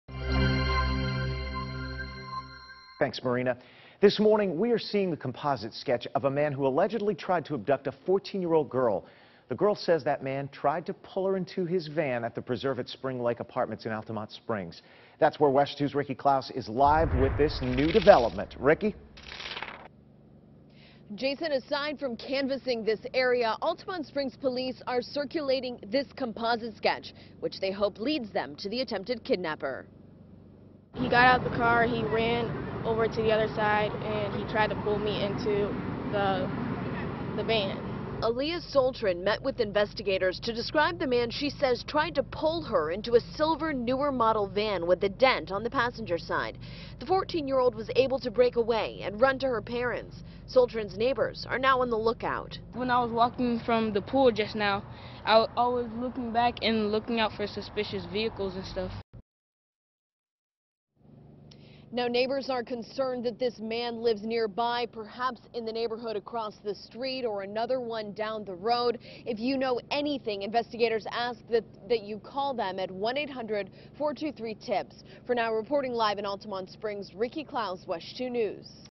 Sketch released in Altamonte Springs attempted abduction
Altamonte Springs police released a composite sketch of a man who tried to abduct a 14-year-old girl at the Preserve at Spring Lake Apartments off Wymore Road on Saturday morning.